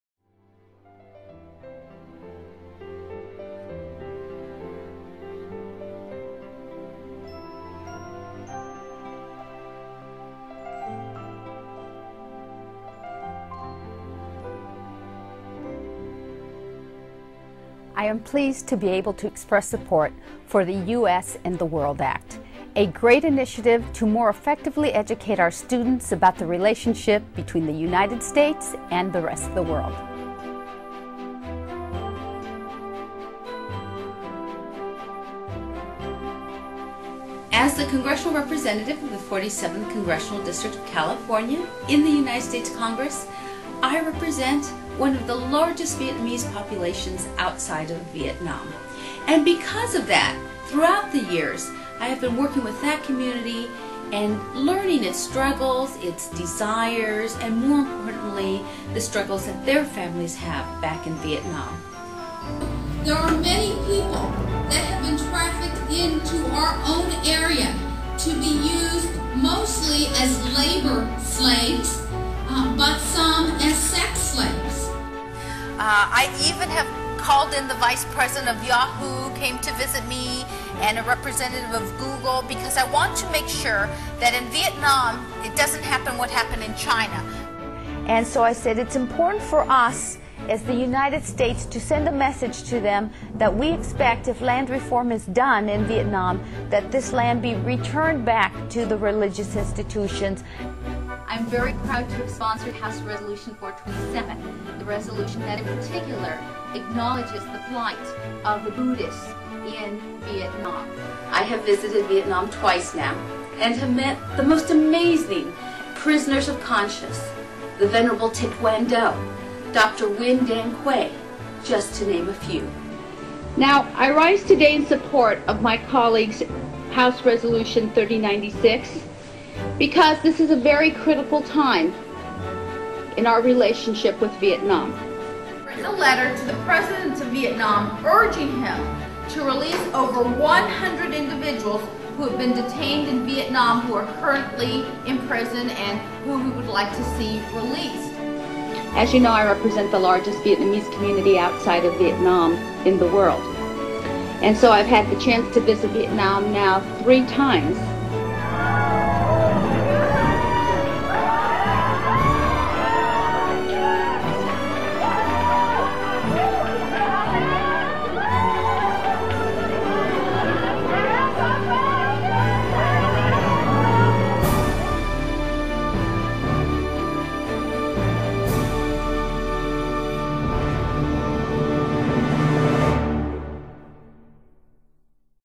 Loretta Sanchez A Friend to the Vietnamese Community
Check out why US Congresswoman Loretta Sanchez from California is a friend to the Vietnamese Community